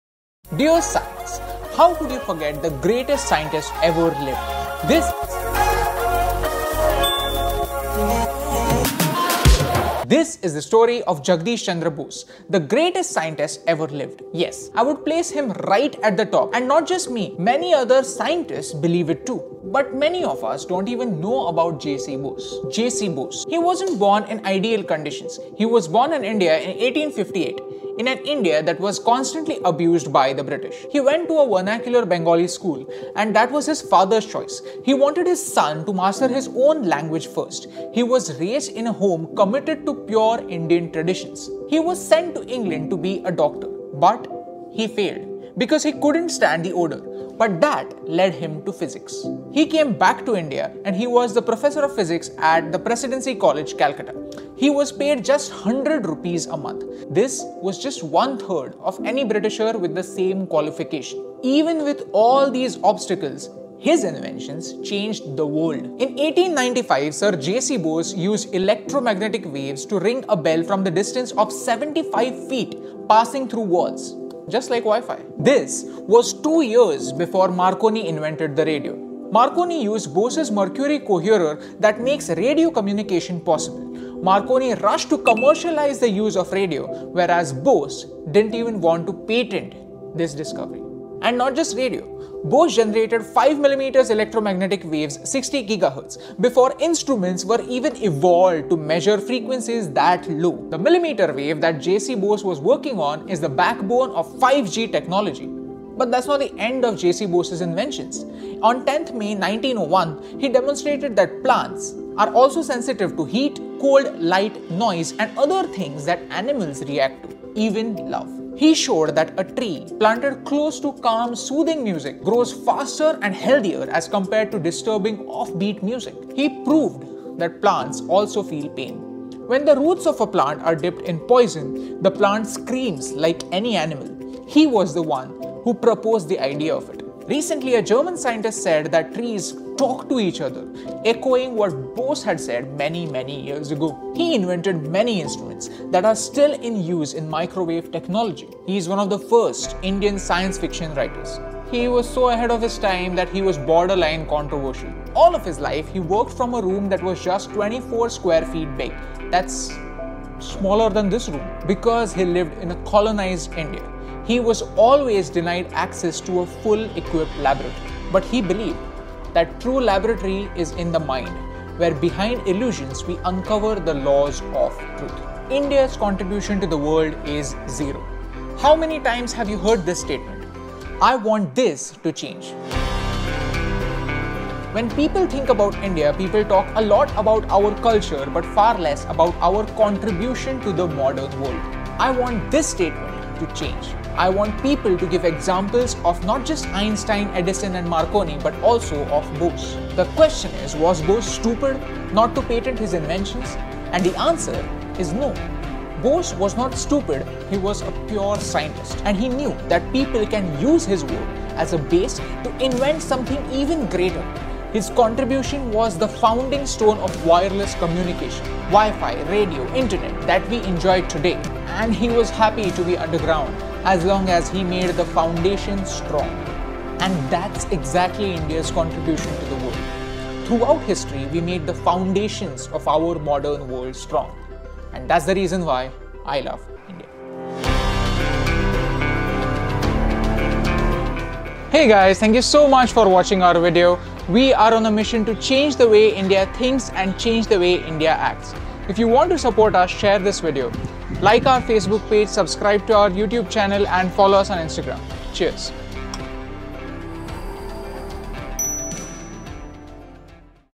Jagdish Chandra Bose - The father of WiFi?
This is the story of Sir J C Bose! Today is his birthday. He was a pure scientist and he was reluctant of registering any patents in his name. He used radio waves 2 years before Marconi invented the radio. And he came up with the revolutionary idea that plants have feelings :)